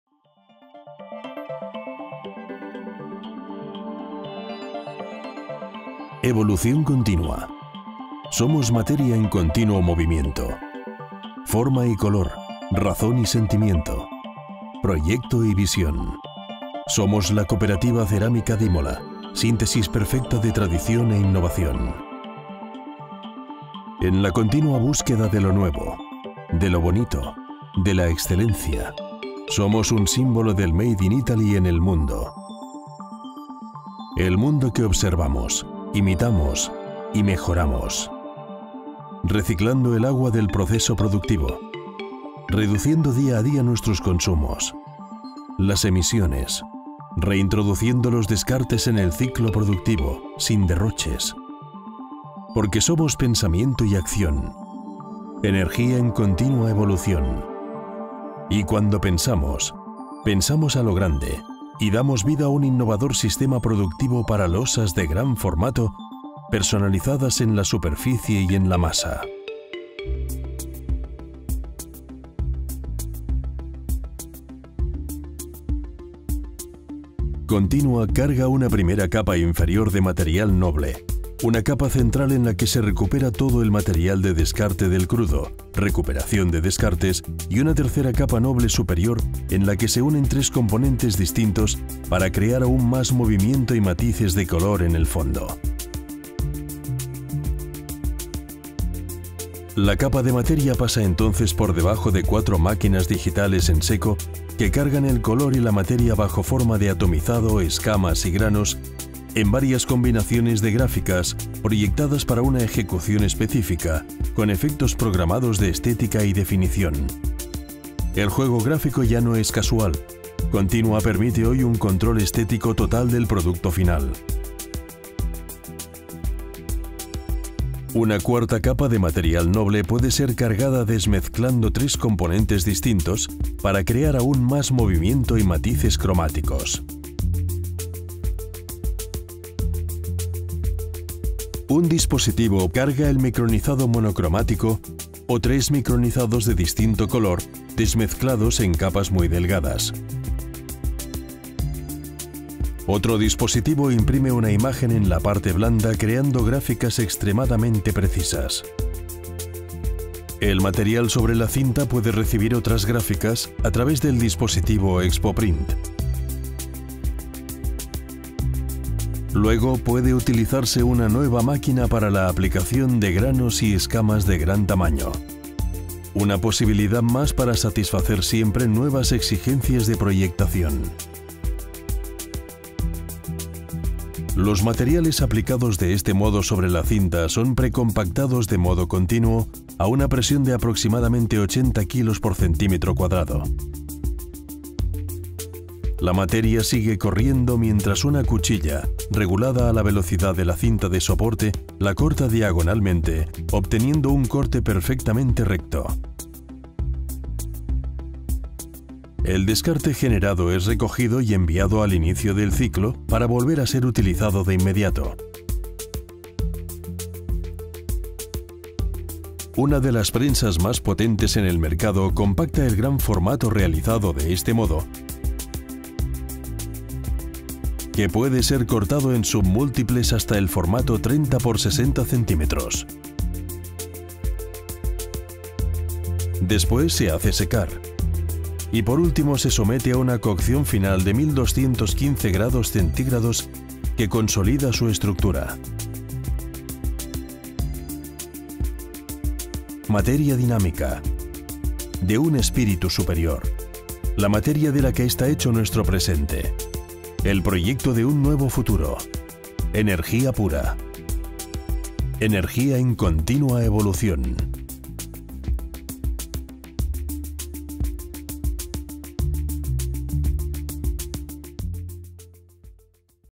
Cooperativa Cermica d'Imola - Continuous Evolution (ES)
CONTINUA is the name of an innovative production system which, in function of completely restructured premises, means that full body porcelainized Grès slabs and tiles can be produced in large format (1.20x1.20 metres), customised in surface and mass.